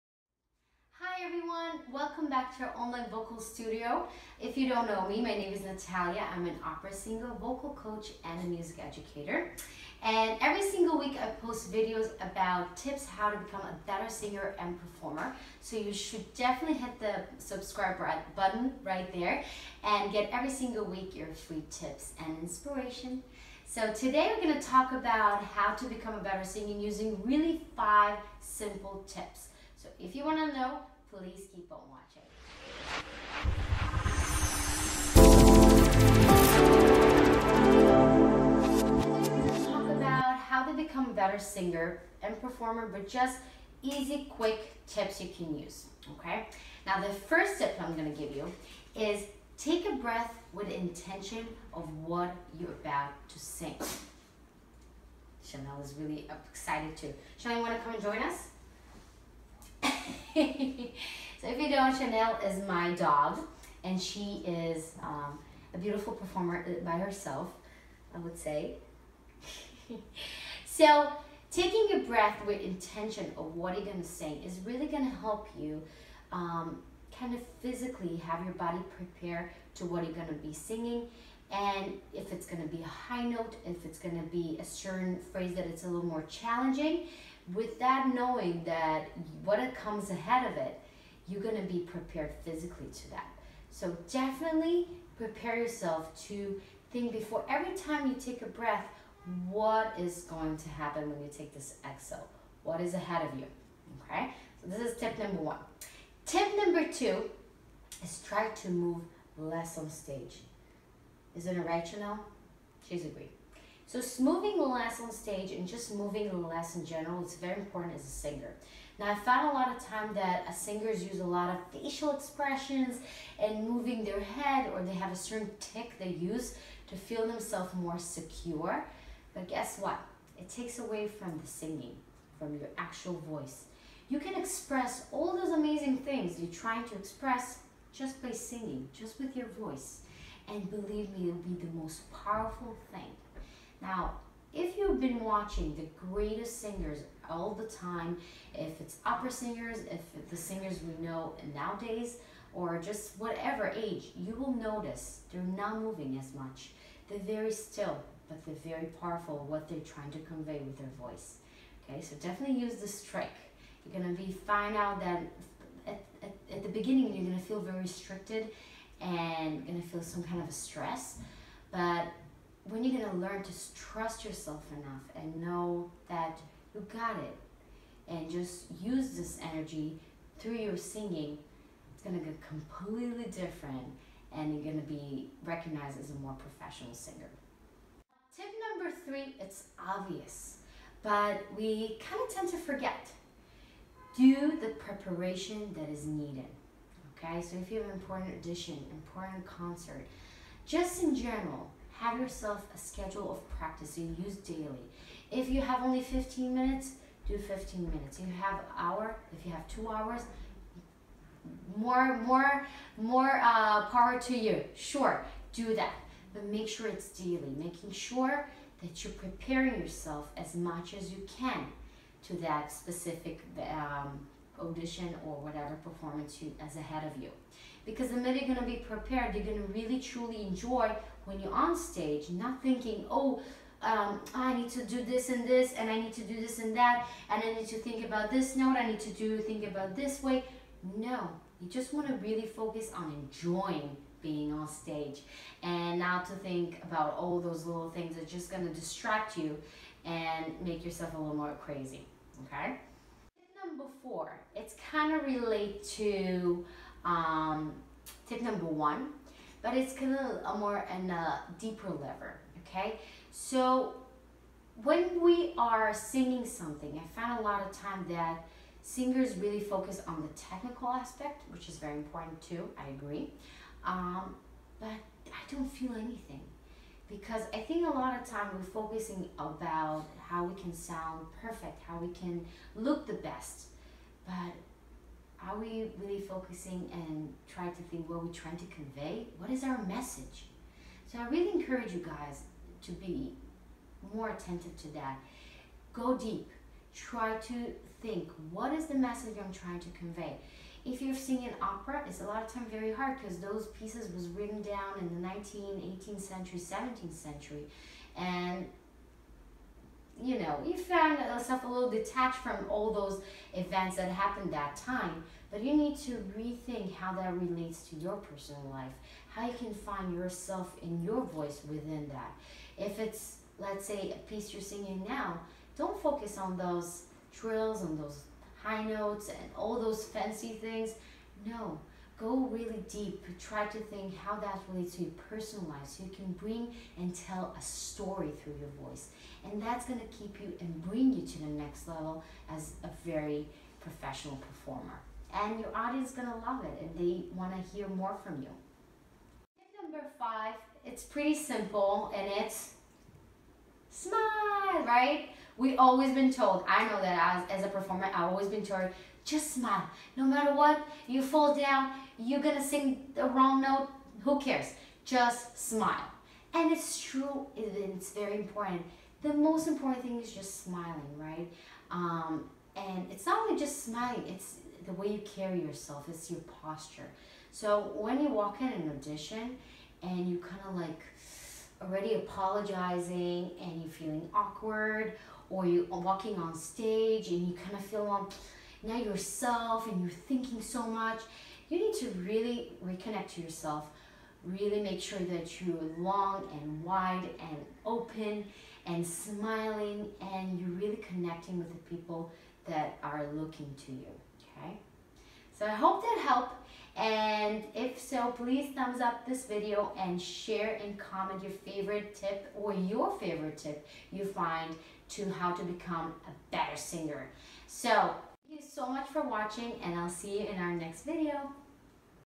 Five Easy Tips to Become a Better Singer NOW! | NATALIA's Online Vocal Studio
In this video, I'll give you my top easy and quick tips for how to sing like a pro right now!